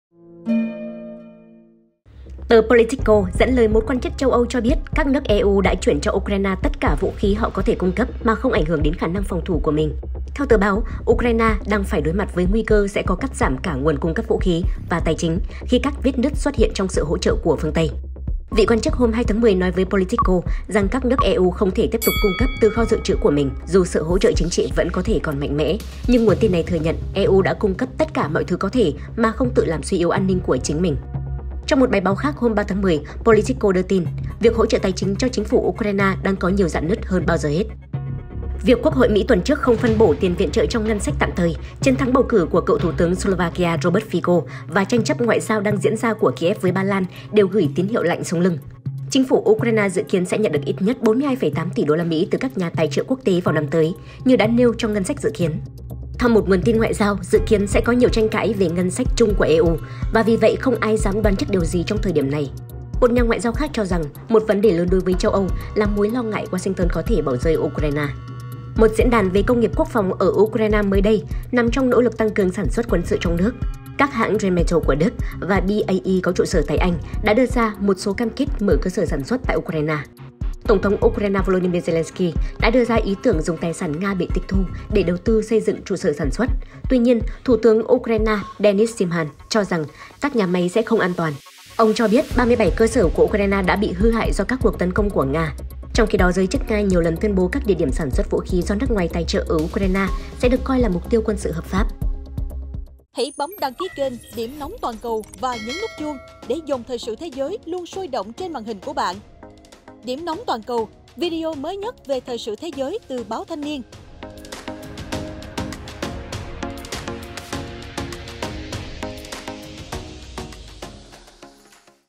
EU đã "vét kho" để cung cấp vũ khí cho Ukraine, khó viện trợ thêm
Tờ Politico dẫn lời một quan chức châu Âu cho biết các nước EU đã chuyển cho Ukraine tất cả vũ khí họ có thể cung cấp mà không ảnh hưởng đến khả năng phòng thủ của mình.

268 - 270 Nguyễn Đình Chiểu, Phường Võ Thị Sáu, quận 3, TP HCM